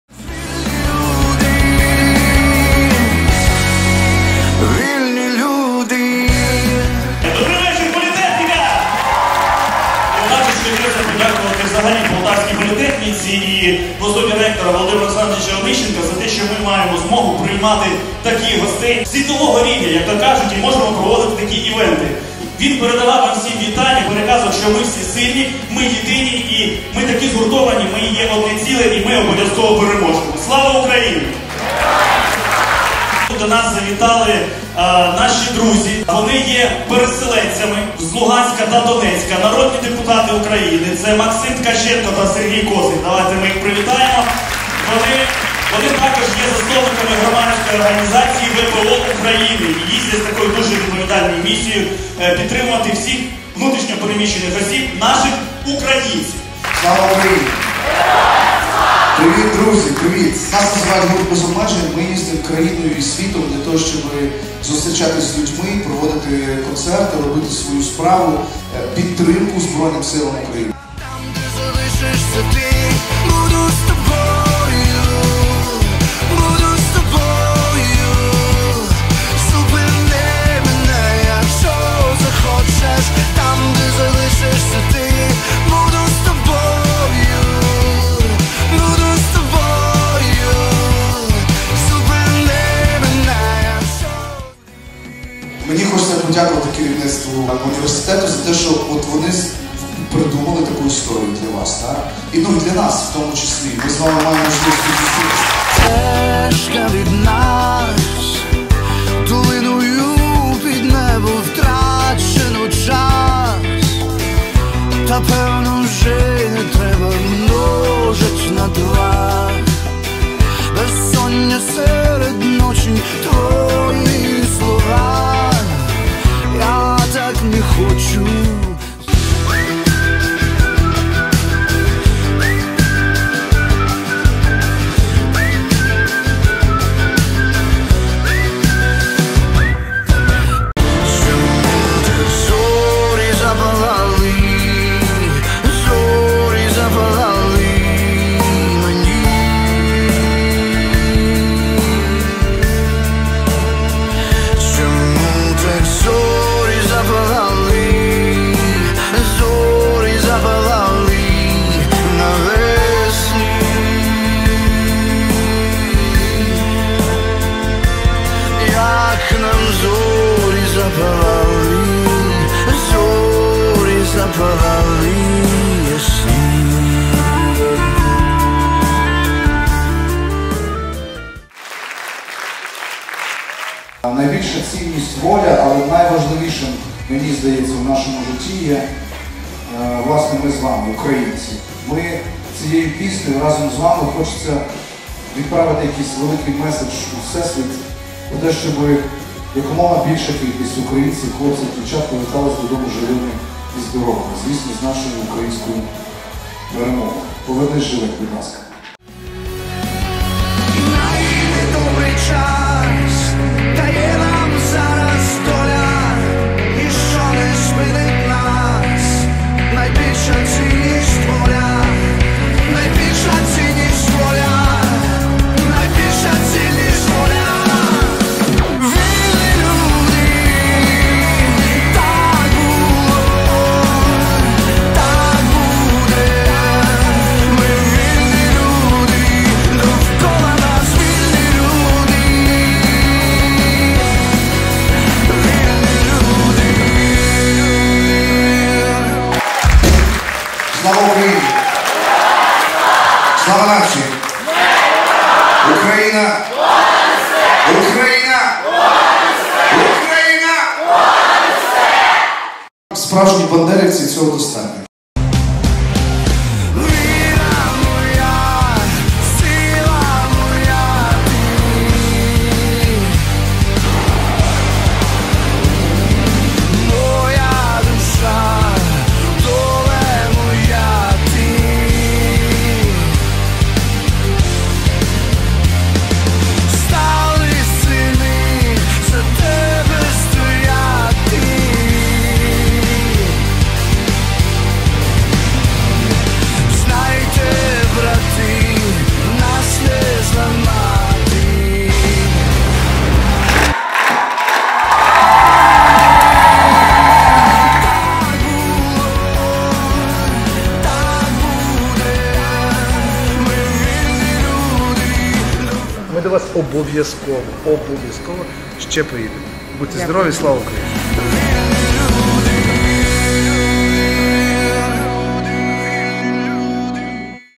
Рок-гурт «БЕЗ ОБМЕЖЕНЬ» зіграв у політехніці благодійний концерт на підтримку ЗСУ
Вперше відомий український рок-гурт «БЕЗ ОБМЕЖЕНЬ» зіграв концерт у закладі вищої освіти – знакова подія відбулась на головній сцені Полтавської політехніки. Рокери провели благодійний концерт на підтримку ЗСУ з програмою «Вільні люди» і подарували публіці як хіти, так і нові пісні, і присвяти сучасним героям.

Викладачі і студенти задонатили 207 706 гривень на придбання автомобіля-позашляховика для потреб захисників.